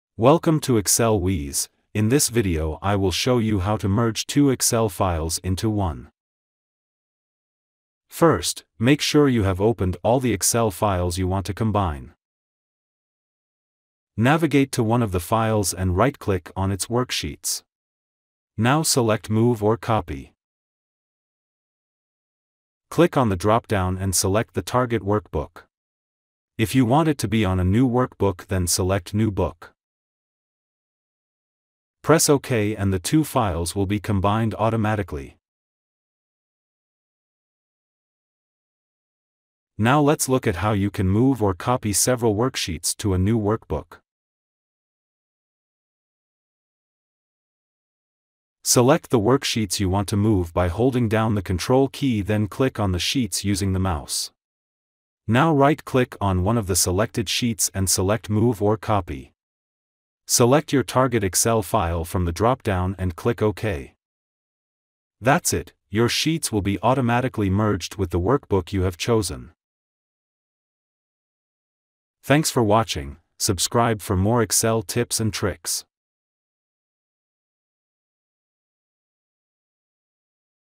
How to Merge two Excel Files into One
In this video, I will show you how to merge two excel files. You will also learn how you can copy or move multiple sheets from one Excel file to another.